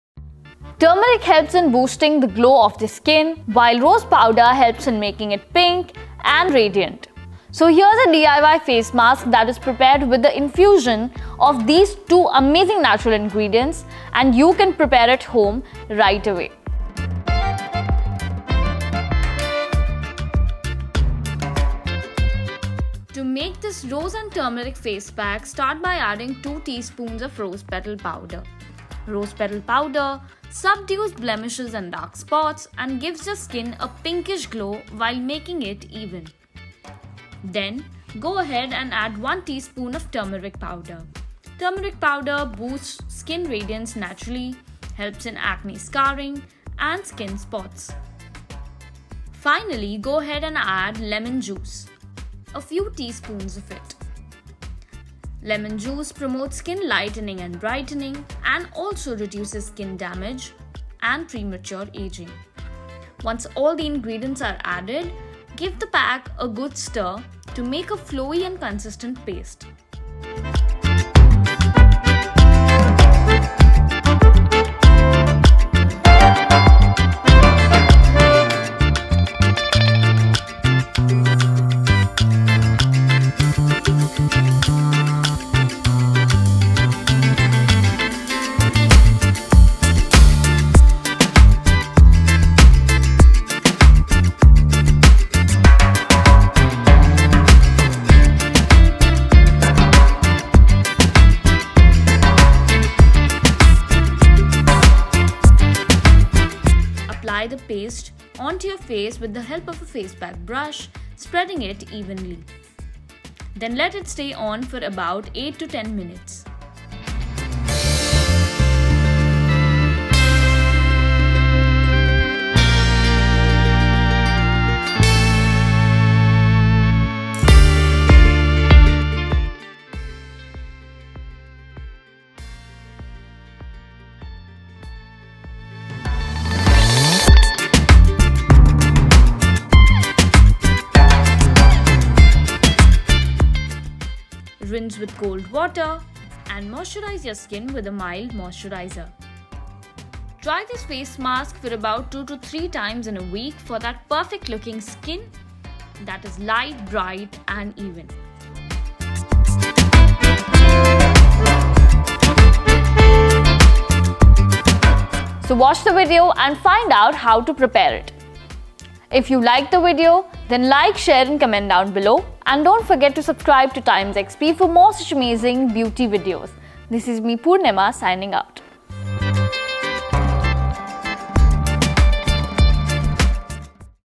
How To Make Rose & Turmeric Face Pack At Home Within 5 Minutes| TimesXP Beauty
Roses are filled with skin healing properties that have a great effect on the skin health overall. And another marvellous ingredient for the skin is turmeric. These amazing natural ingredients boost the skin radiance and make your skin flawless with regular use. So, here we tell you how you can make a face mask with these amazing ingredients right at home within 5 minutes.